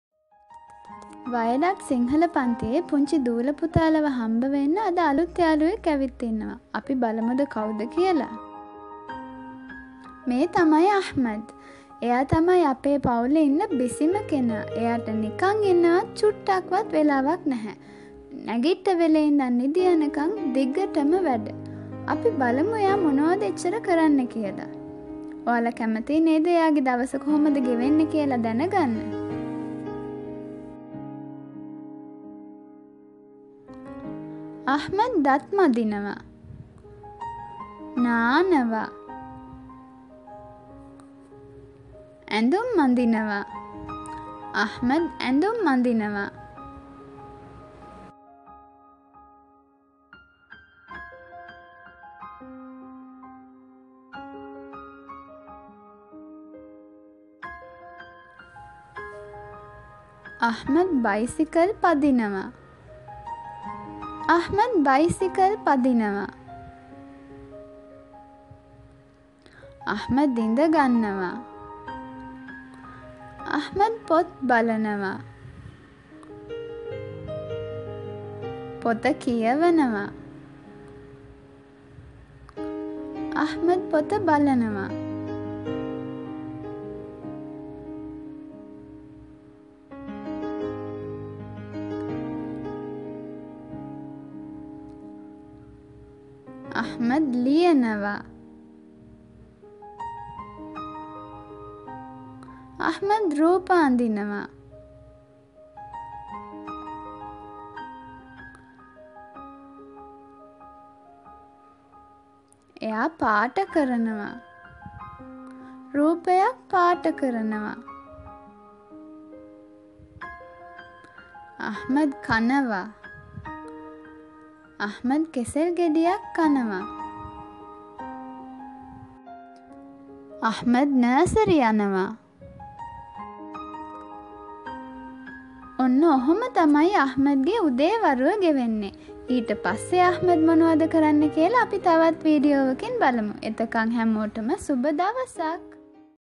Learn verbs in Sinhala with Ahmed - part 01
This  is a series designed to teach kids verbs in Sinhala together with their past tense . The lessons will be continued  in the future vedios